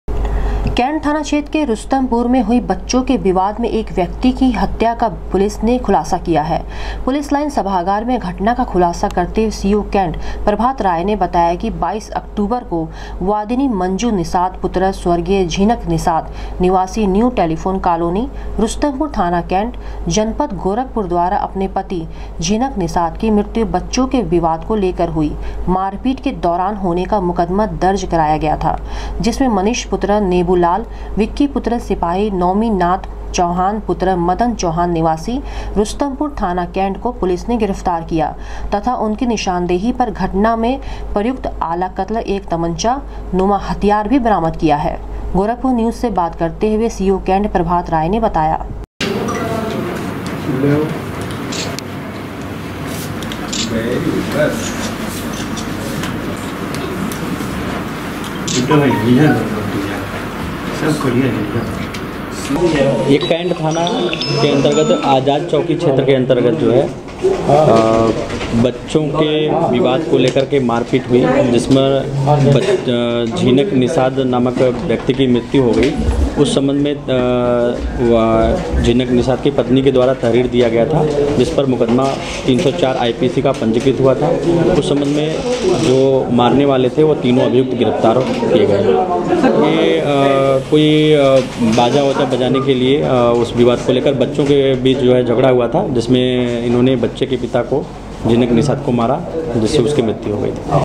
कैन्ट थाना क्षेत्र के रुस्तमपुर में बच्चों के विवाद के दौरान हुई मारपीट के हत्यारोपी को पुलिस
कैन्ट थाना क्षेत्र के रुस्तमपुर में बच्चों के विवाद के दौरान हुई मारपीट के हत्यारोपी को पुलिस ने किया गिरफ्तार, सीओ कैन्ट ने किया खुलासा